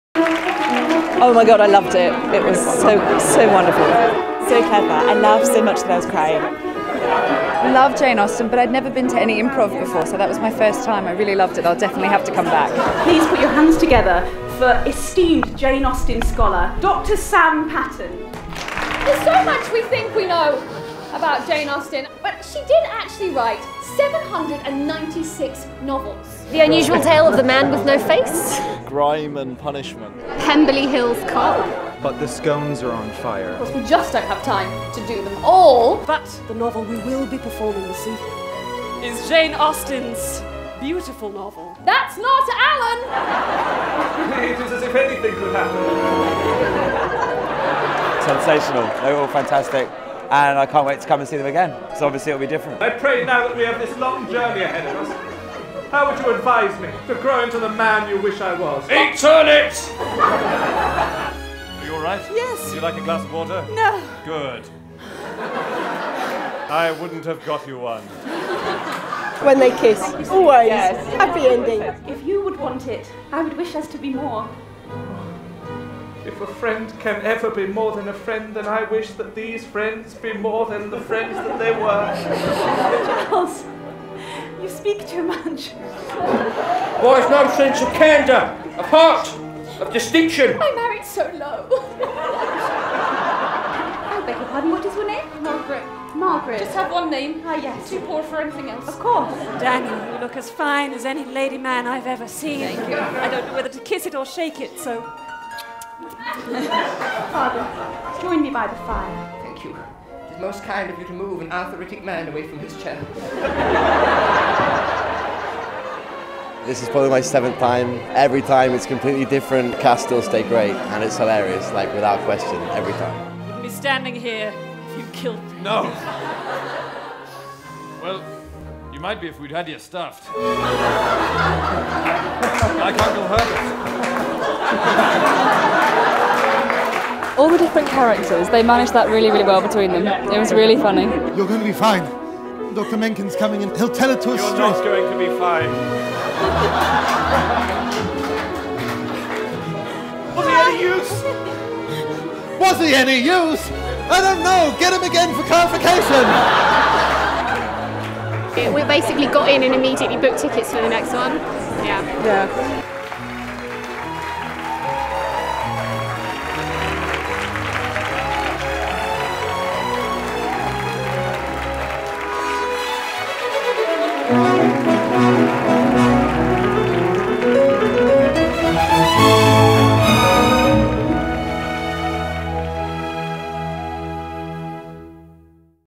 Austentatious | Brighton Dome | Sept 19 Oct 2021
Austentatious is saddling the horses for a Grand Tour of the United Kingdom!

The improvised comedy play returns to Brighton Dome, starring a cast of the country’s quickest comic performers. Every single show the cast conjures up a brand new ‘lost’ Jane Austen novel based on nothing more than a title suggested by the audience.